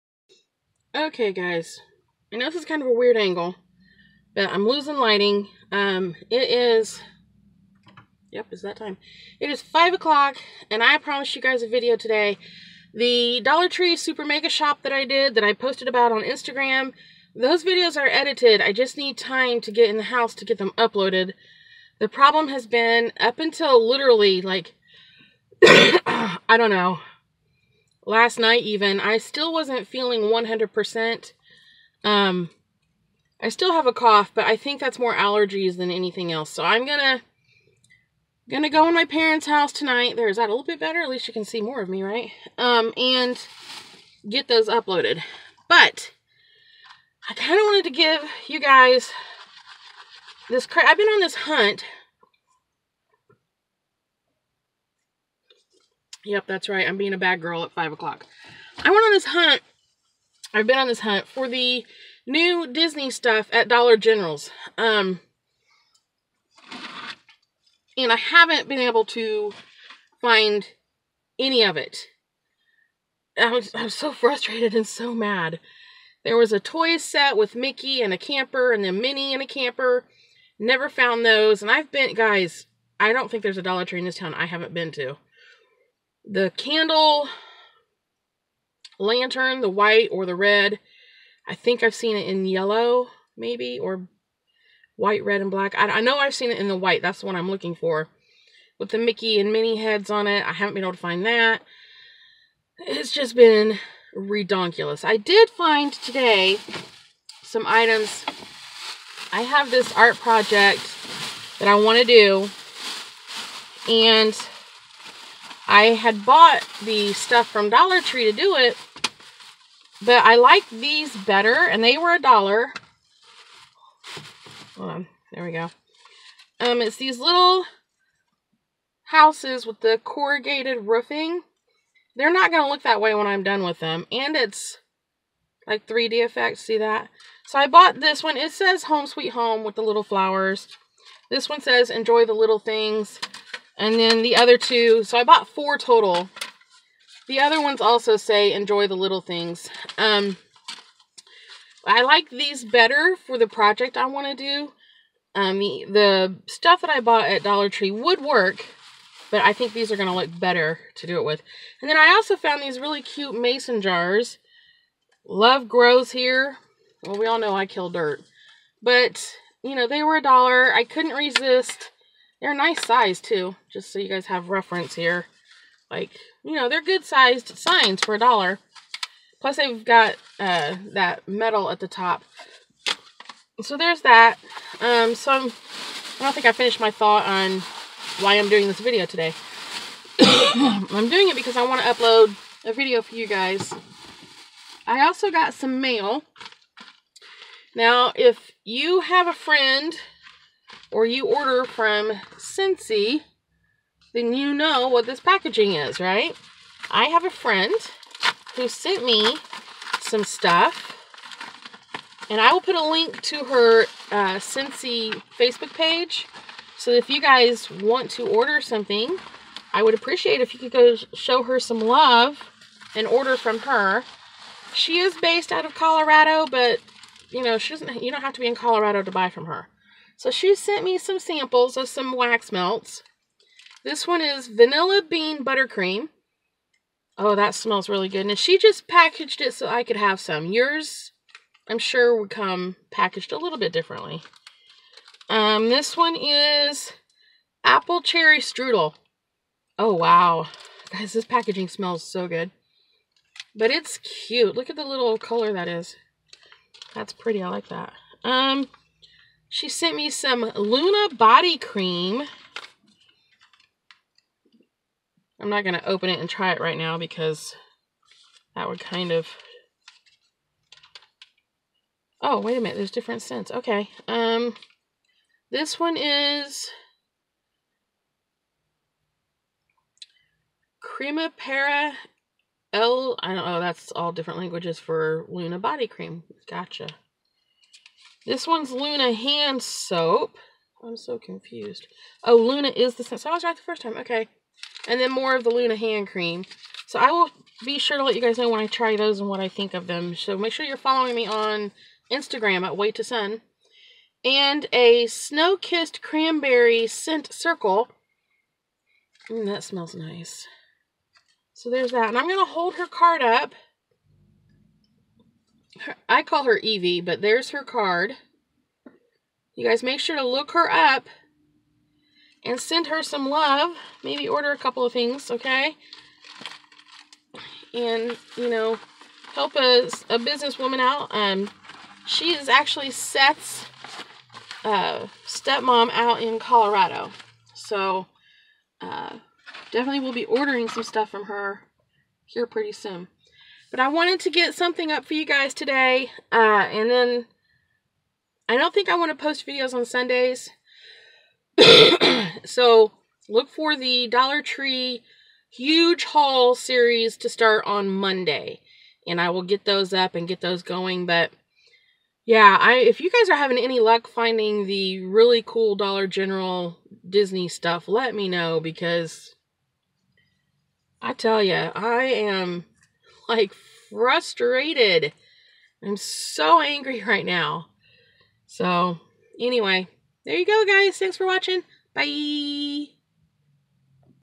Friend Mail and Whats Up!!!??!!
Thank you all for your patience...   here is a quick little haul for ya from Dollar General and some friend mail... go check out Evie (Yvonne) on facebook. Here is her page..